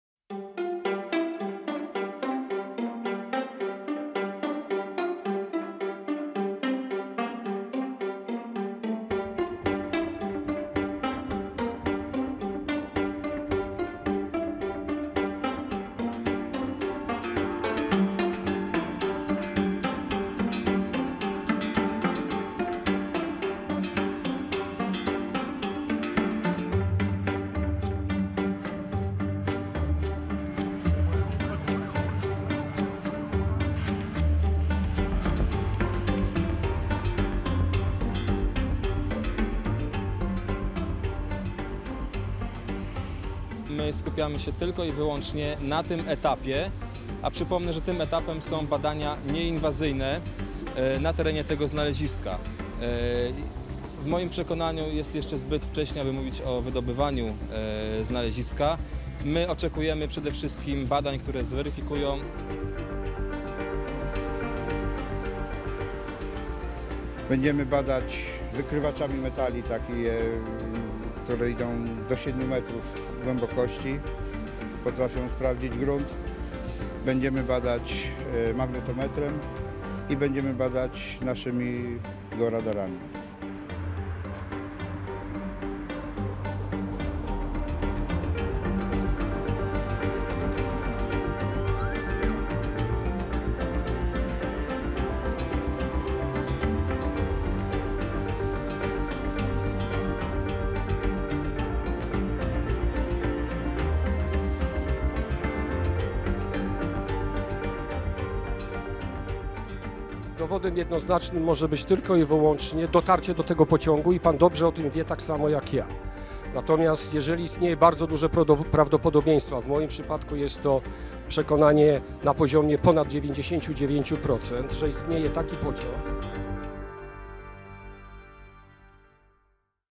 Search For 'Nazi Treasure Train' Set To Begin
November 10, 2015 - Searchers in Poland have been granted permission to start a non-invasive inspection of the site of an alleged Nazi "gold train" from WWII.